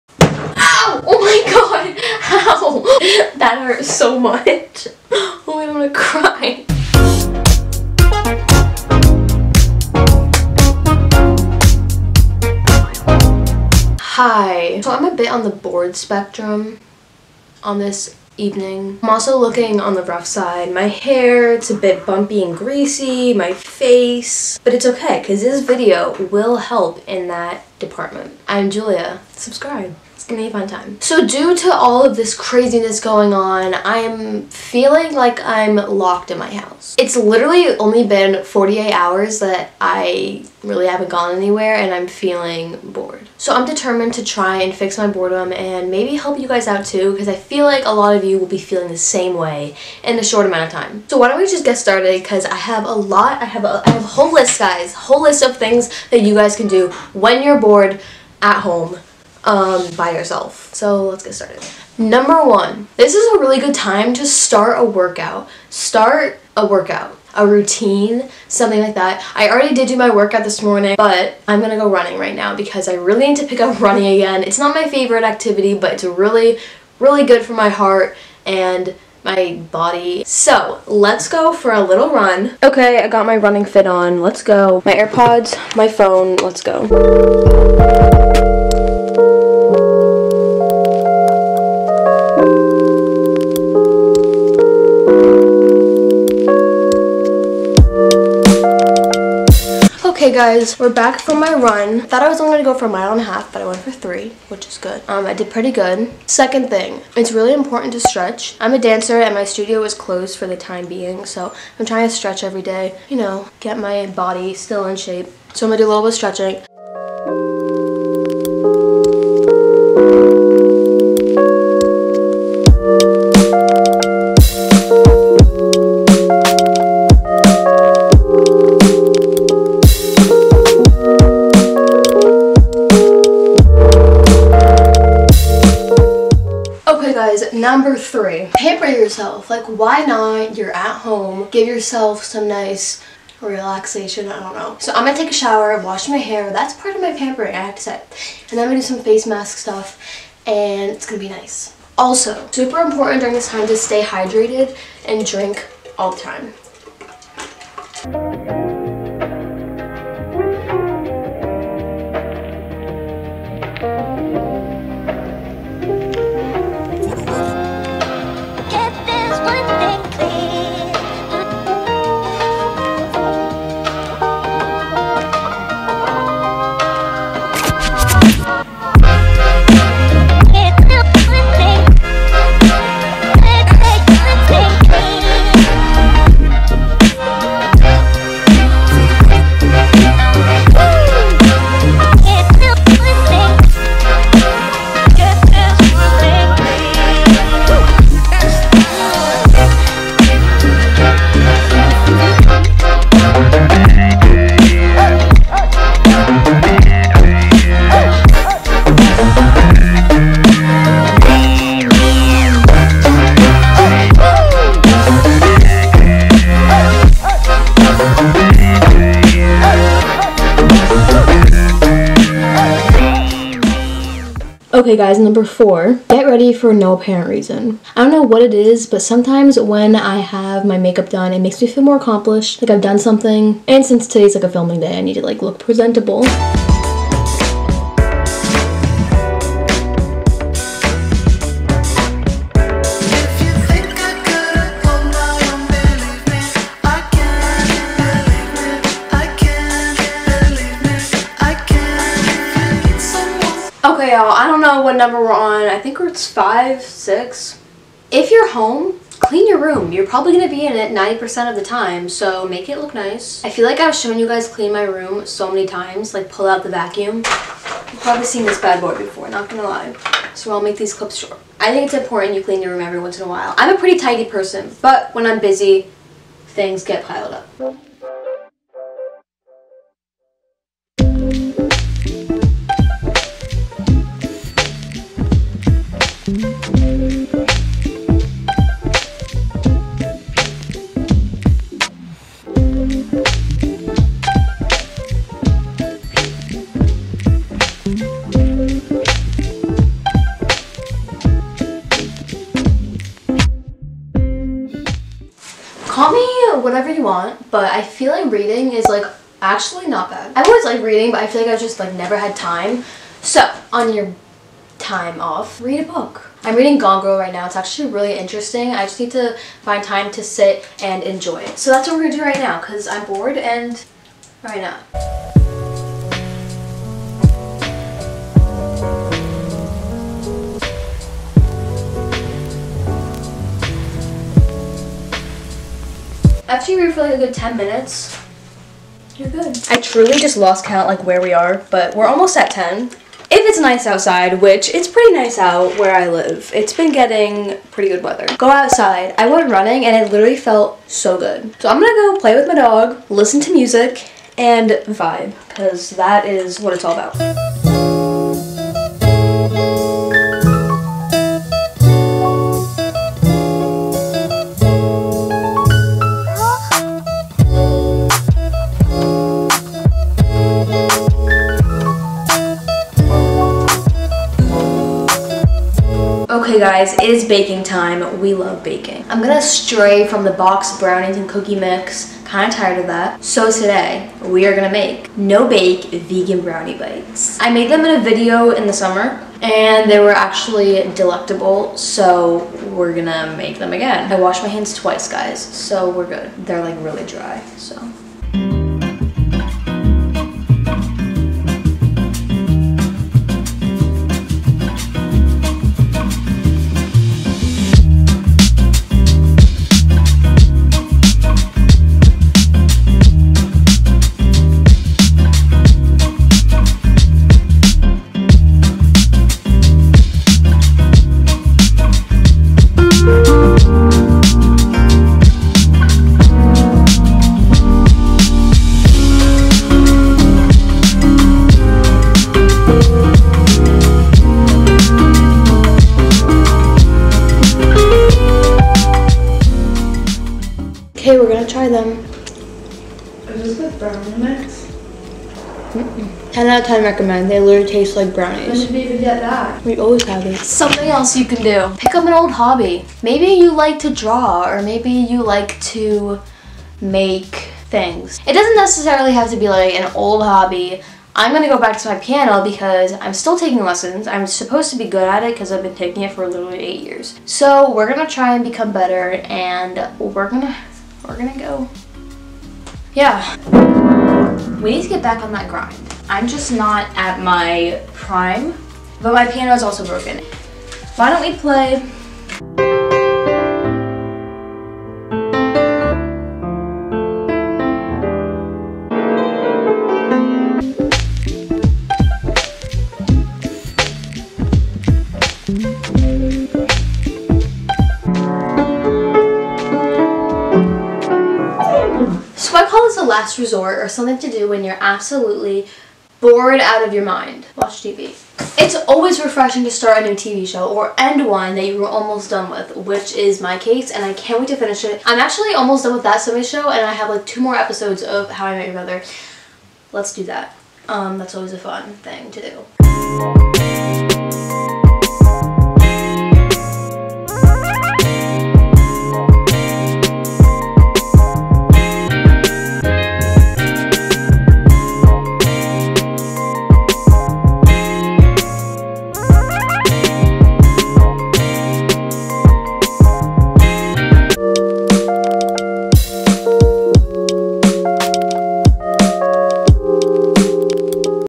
10 things to do when you are bored at home... alone
1o things to do when you have literally nothing to do:
lol i hope you guys enjoy this video, and I hope it cures your boredom in some way.
Let me know if this video helped, or if you tried anything!
Love you all, stay safe and healthy!
**recipe for vegan brownie bites:**

FAQ:
What camera do I use? Canon EOS rebel T6
What editing software do I use? iMovie:):)